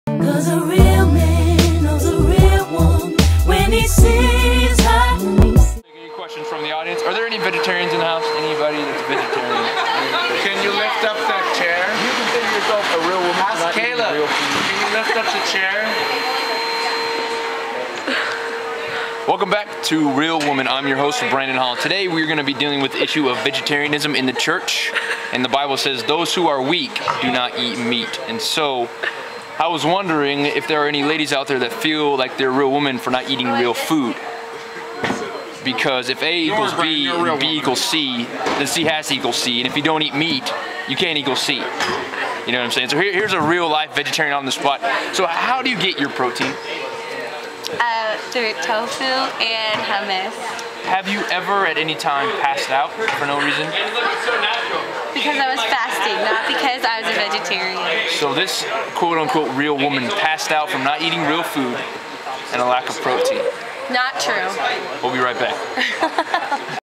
Real Woman, Real Problems 1
During an intern mission trip to las vegas one of the ministries of the church there was called real woman. So i decided to start my own video series addressing the many problems woman face today. Enjoy!